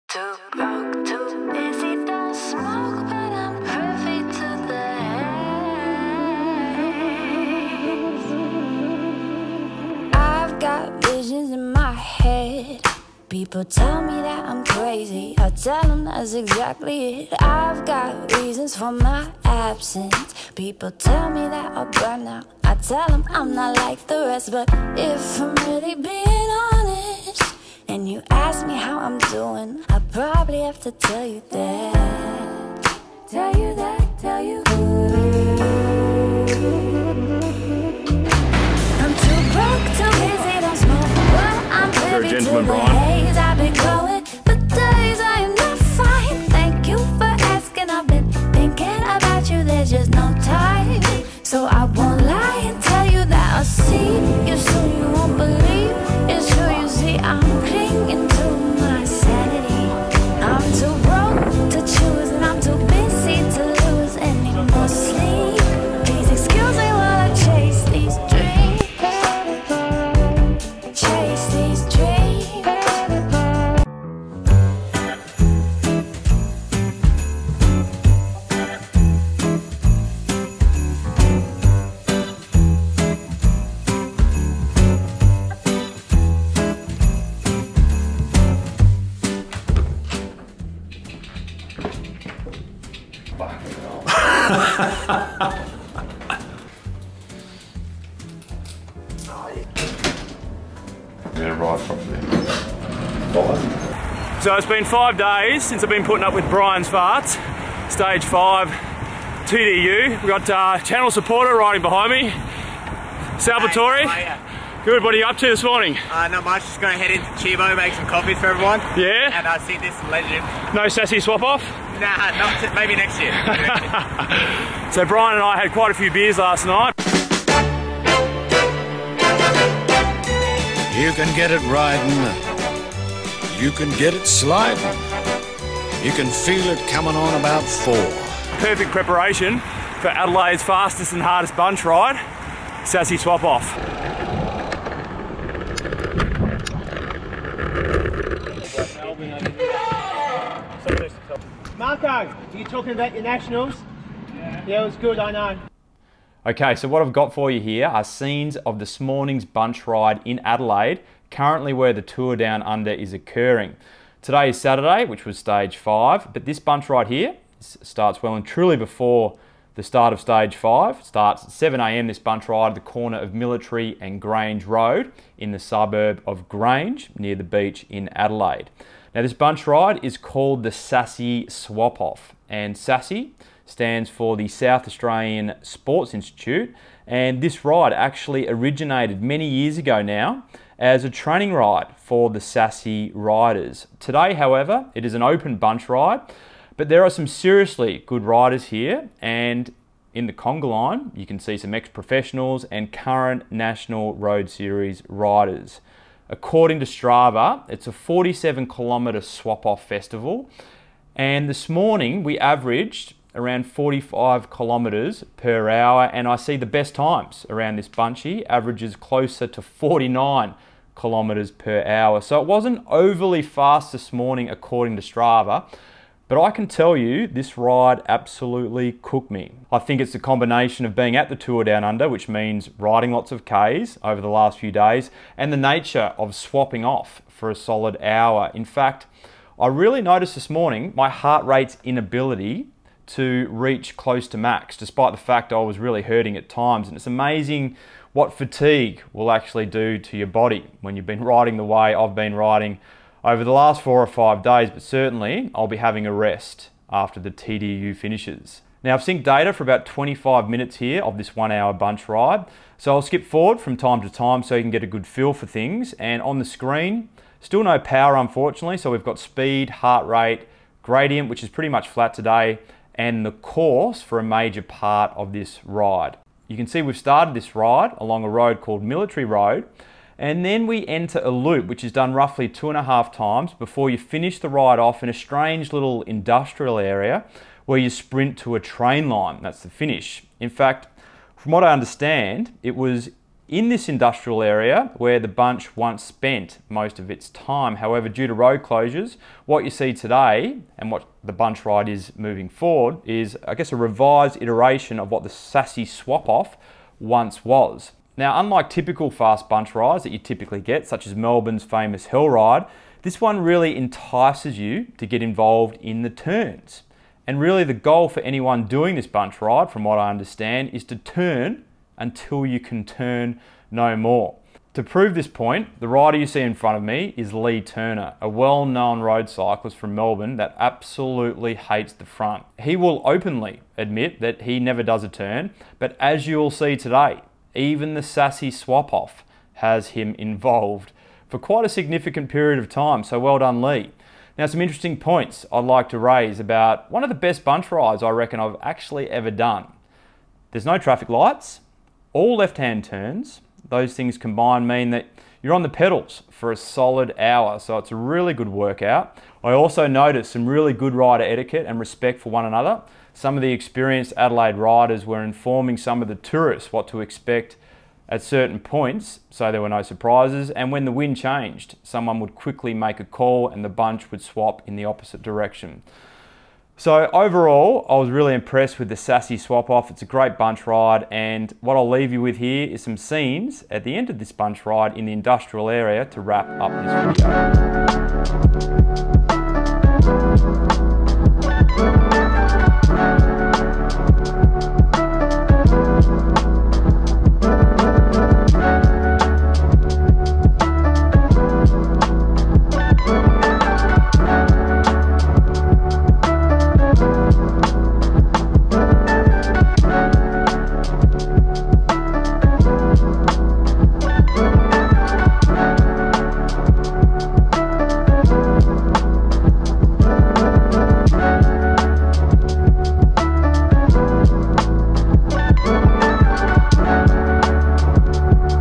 Adelaide's Fastest & Hardest Bunch Ride (Tour Down Under Style)
In this video, we are Stage 5 of the Santos Tour Down Under, however, this is what the tourists are doing pre-road race. Adelaide's most famous Saturday bunch ride. I take the BMC Teammachine to one of the fastest and hardest bunch rides I have ever done. The SASI swap off. I also share some scenes from the Tour Down Under so far and provide an overview of this famous bunch ride.